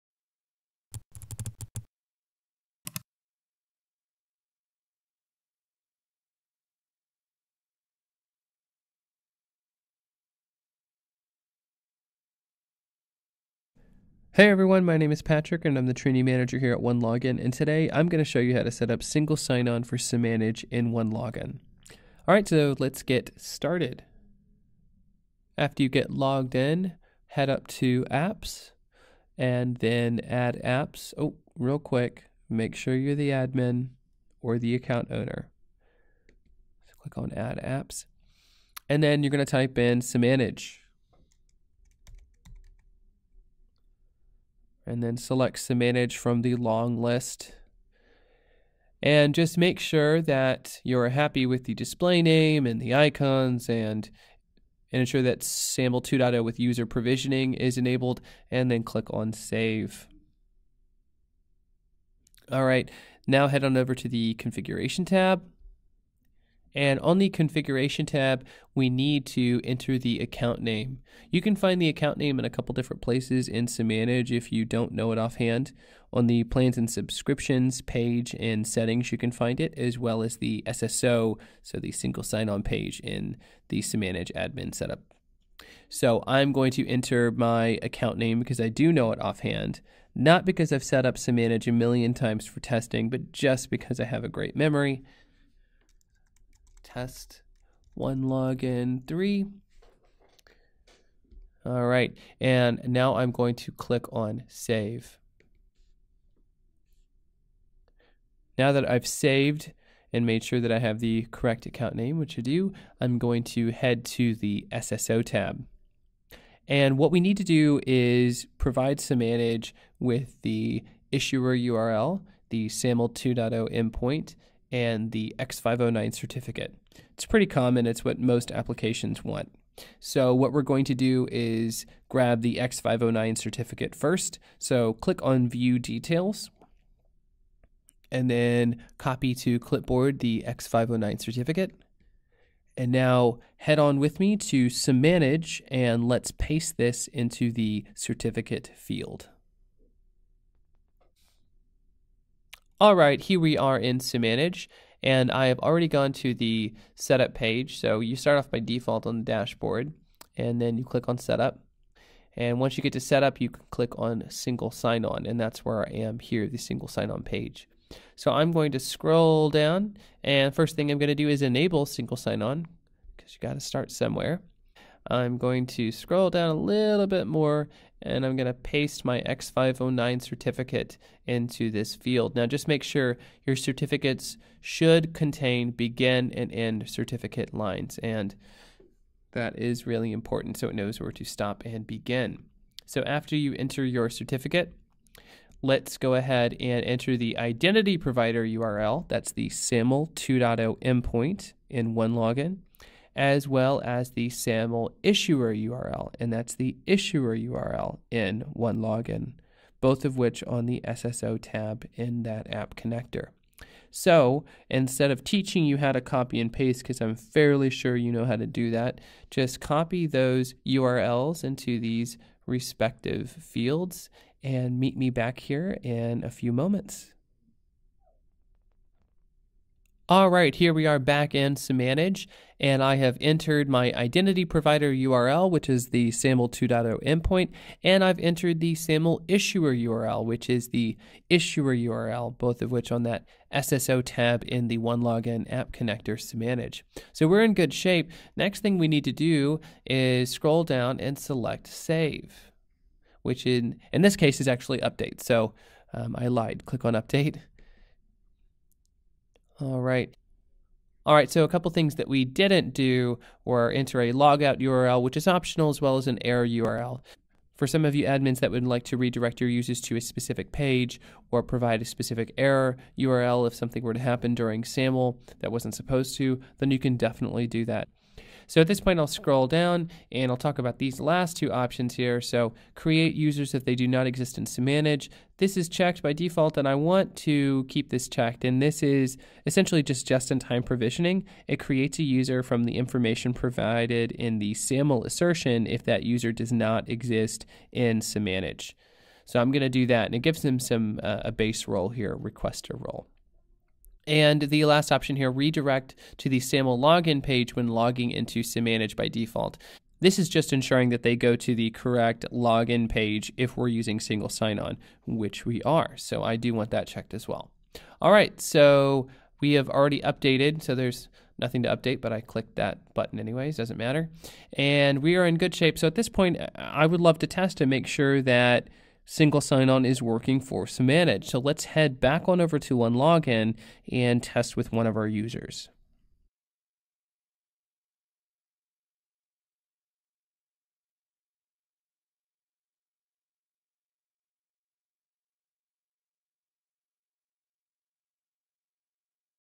How to set up Single Sign-On for Samanage (Part 1 of 2) | OneLogin
This video tutorial covers how to setup (SAML) Single Sign-On and just-in-time provisioning for Samanage.